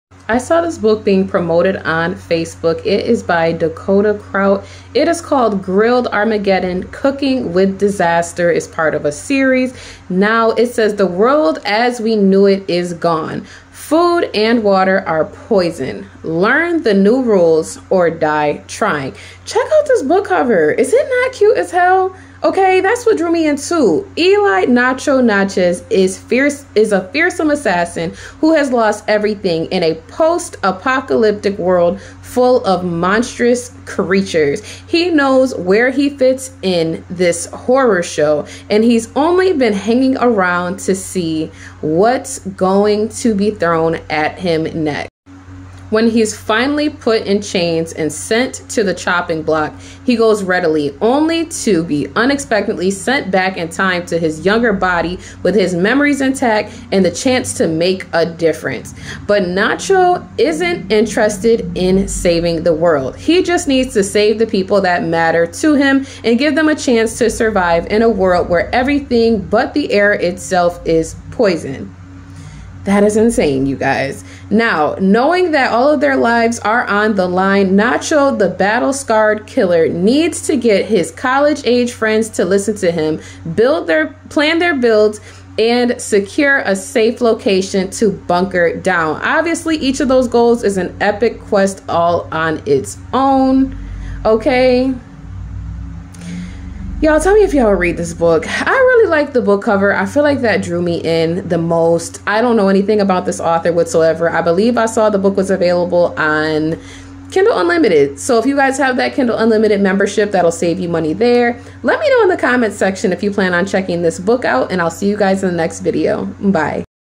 Cooking with Disaster 🥣🥢 Grilled Armageddon
Cooking with Disaster Book 1

Dakota Krout

Grilled Armageddon (Cooking with Disaster Book 1

The world as we knew it is gone. Food and water are poison. Learn the new rules, or die trying.

Eli ‘Nacho’ Naches is a fearsome assassin who has lost everything in a post-apocalyptic world full of monstrous creatures.

He knows where he fits in this horror-show, and he’s only been hanging around to see what’s going to be thrown at him next.

When he’s finally put in chains and sent to the chopping block, he goes readily… only to be unexpectedly sent back in time to his younger body with his memories intact, and the chance to make a difference.

But Nacho isn’t interested in saving the world. He just needs to save the people that matter to him, and give them a chance to survive in a world where everything but the air itself is poison.

 Knowing that all their lives are on the line, Nacho the battle-scarred killer needs to get his college-age friends to listen to him, plan their builds, and secure a safe location to bunker down.

Obviously, each of those goals is an Epic quest all on its own.